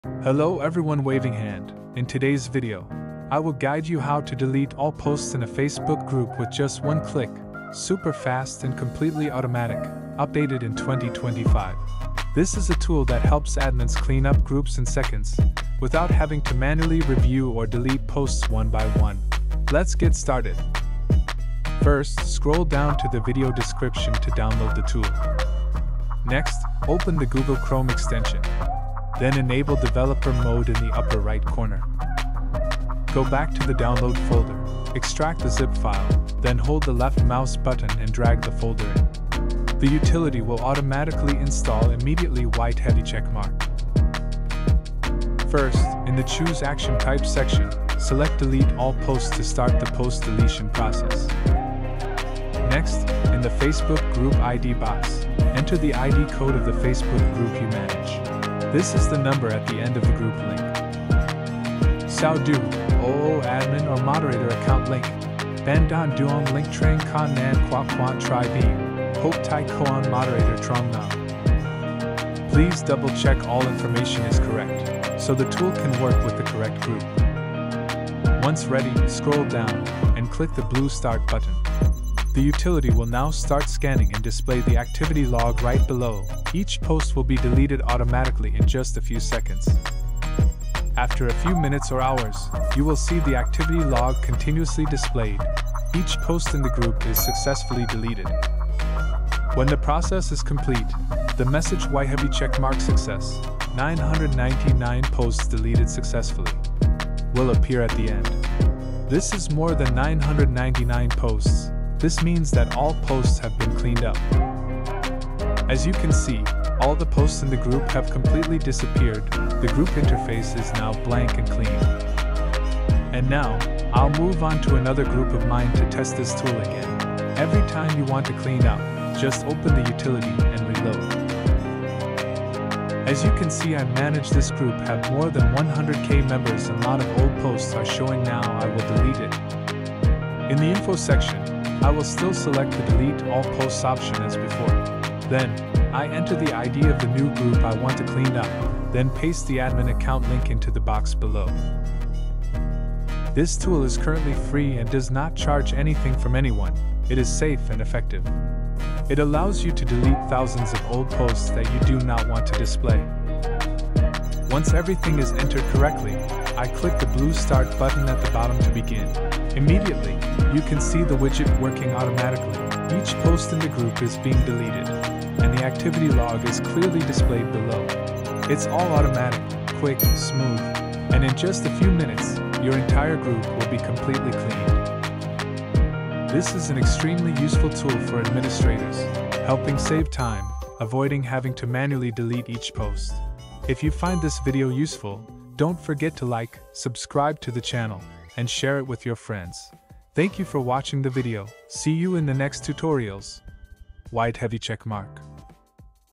How to Delete All Posts on Facebook Group in One Click (2026) | Delete All Group Posts Easily Fast
In this tutorial, you’ll learn how to delete all posts in your Facebook group in one click (2026 update) — fast, easy.

🎯 If you’re a Facebook group admin managing a large community with hundreds or even thousands of posts, this tutorial is perfect for you.
By following this simple process, you can remove all group posts instantly, clean up your group’s feed, and make it look brand new again.

🔥 Learn the easiest way to delete all posts in Facebook group, remove spam automatically, and manage your Facebook community more efficiently.

What You’ll Learn:
🔹 How to delete all Facebook group posts automatically in seconds
🔹 Step-by-step process to clean up your group quickly
🔹 Works perfectly on the latest Facebook layout (2026 version)
🔹 How to manage your Facebook group more efficiently

💡 Best For:
🧩 Facebook group admins who want to reset or refresh their group
🧩 Creators and business owners managing large Facebook communities
🧩 Moderators who want to clean old content quickly
🧩 Anyone looking for a simple way to delete all group posts instantly